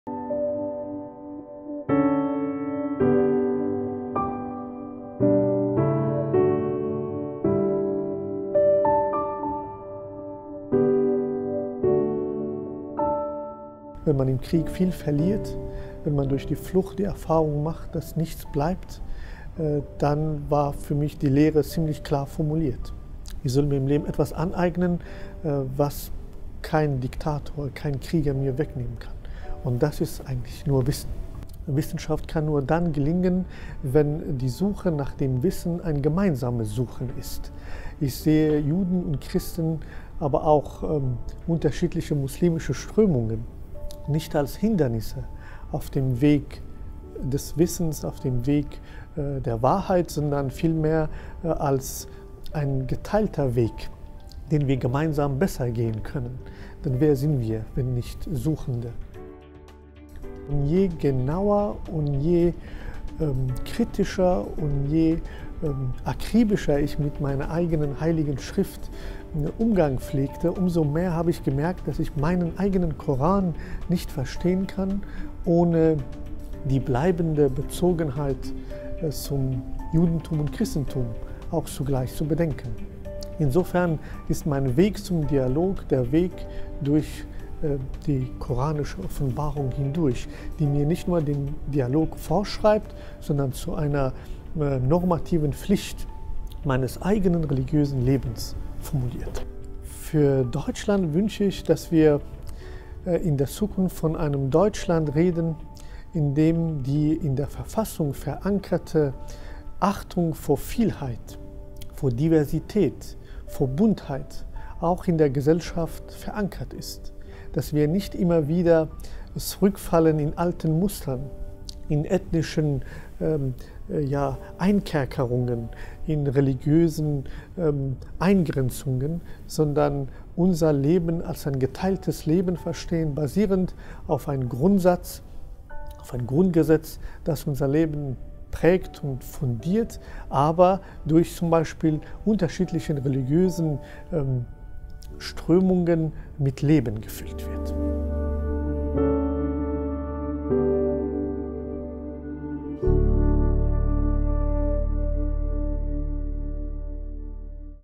Prof. Dr. Ahmad Milad Karimi – Kategorie: Wissenschaft & Bildung
Prof. Dr. Karimi ist ein Wegbegleiter des Dialogs, der sich der Wichtigkeit der Begegnung bewusst ist. Er hat den interreligiösen Dialog insbesondere in der Spiritualität geprägt, und hat seinen Mitmenschen bewiesen, dass es im Dialog wichtig ist, nicht nur das Verbindende zu thematisieren, sondern auch das Trennende, um sich näher zu kommen – um gemeinsam den Pfad des Dialog beschreiten zu können.

Durch seine Erfahrung hat Prof. Dr. Karimi Andere dazu inspiriert, mit offenem Herzen ihr Gegenüber zu betrachten und die einzigartige Erfahrung des aufrichtigen Dialogs zu erleben.

„Sich im Herzen des Anderen zu begegnen“ – Dank seiner Arbeit konnten viele Menschen, die sich dem Dialog gewidmet haben, eine tiefere Bindung zu ihren DialogpartnerInnen aufbauen.

Deutscher Dialogpreis 2019